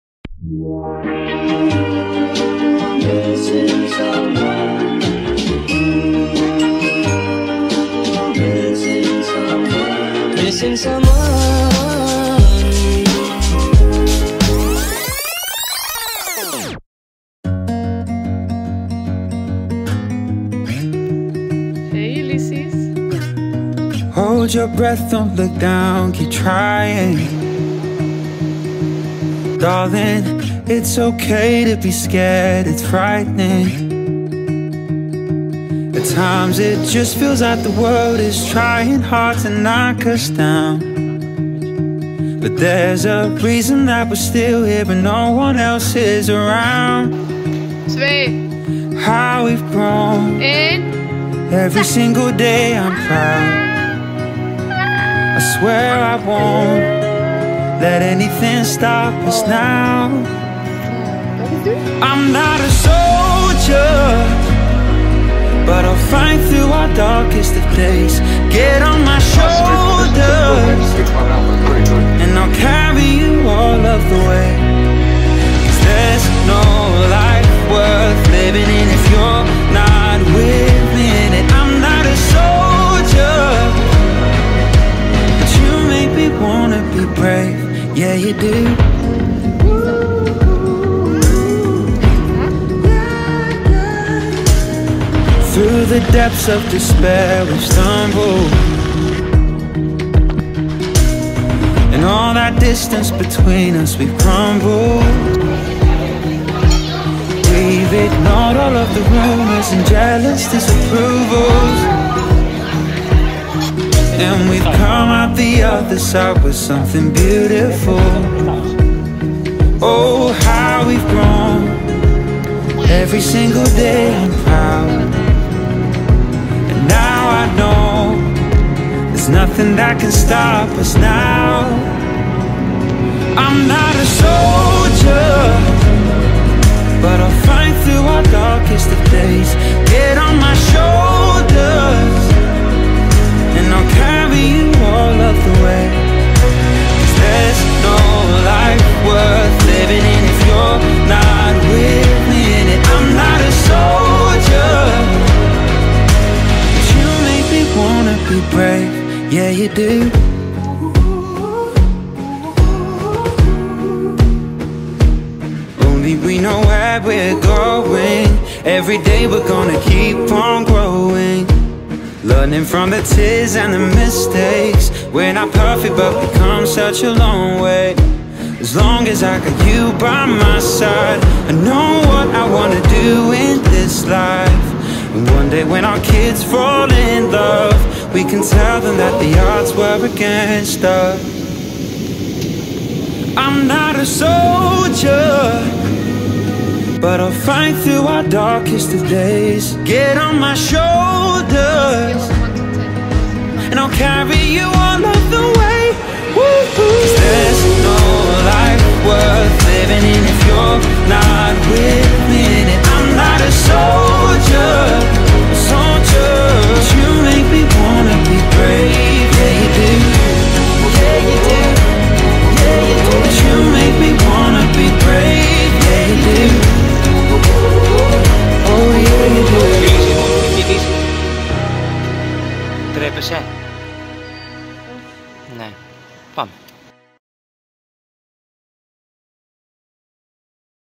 Travel week from New Zealand to Phuket: 4 flights in 7 days with a 18 month old!
We made it in Phuket!
It took us 7 days and 4 flights but we made it ;) We said goodbye to Australia and New Zealand and are now getting over our jetlag in Phuket.
Big hugs
Ulysses, Michalis and Aurelie